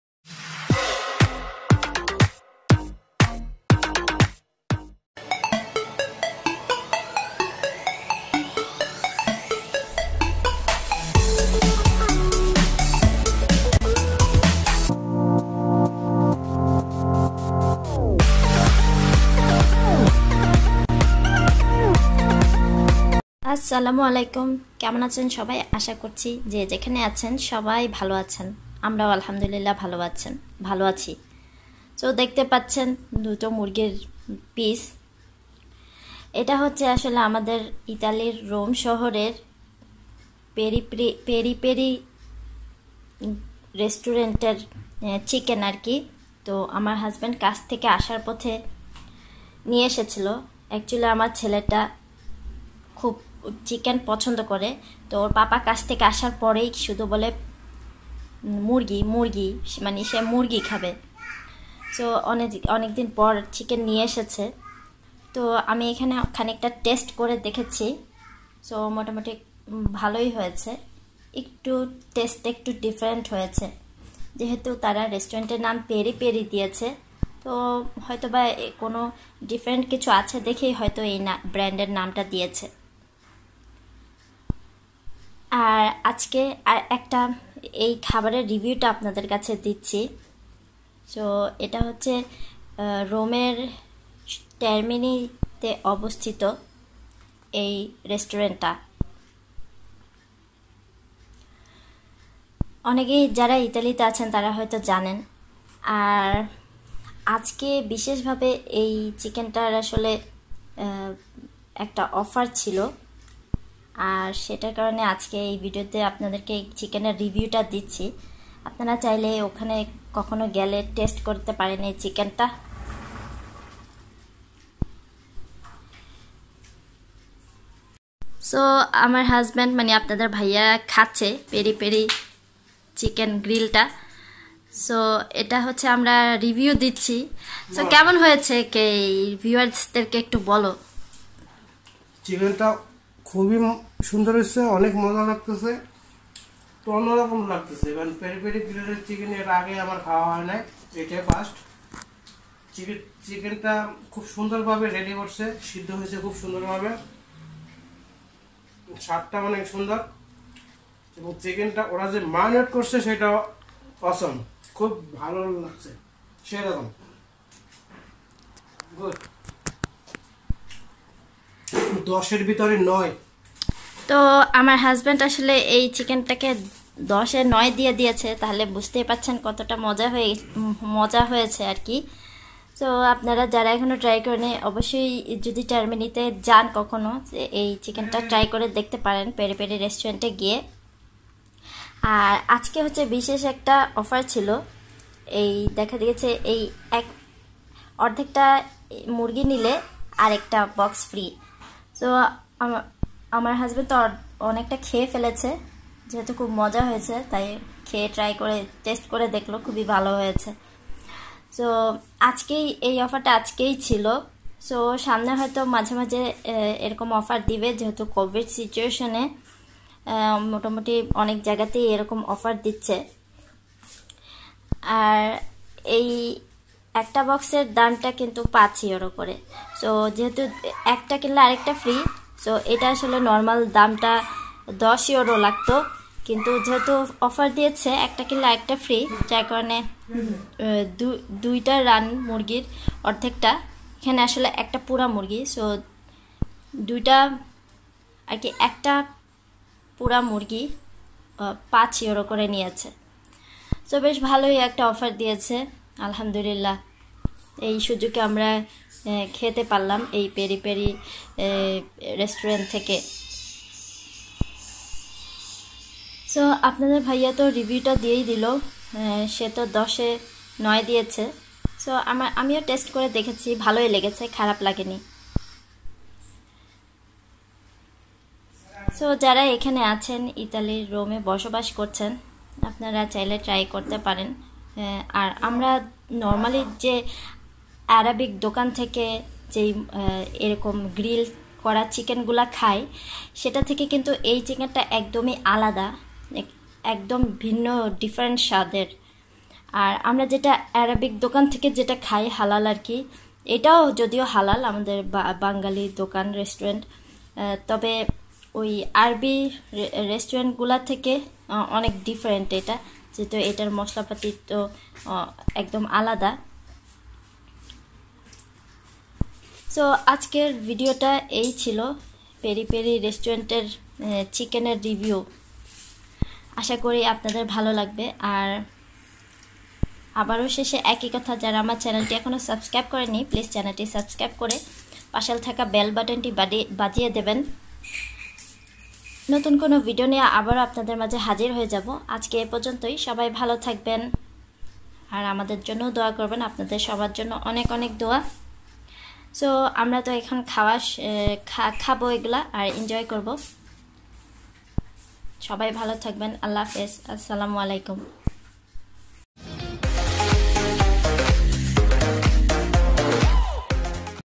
Peri Peri Chicken Review | Santa Chowdhury vlogs
Peri Peri chicken Review

ইতালিতে রোম শহরে একটি পেরিফেরি রেস্টুরেন্ট রয়েছে

সেখানে প্রায় সময় খাবারের অফার দেয় আজকের এই চিকেন টা রিভিউ দিলাম অনেক ভালো হয়েছে আপনারা যারা ট্রাই করতে চান অবশ্যই সেখানে গিয়ে পেরিফেরি রেস্টুরেন্টের চিকেন টা ট্রাই করতে পারেন আশা করি আপনাদের ভালো লাগবে|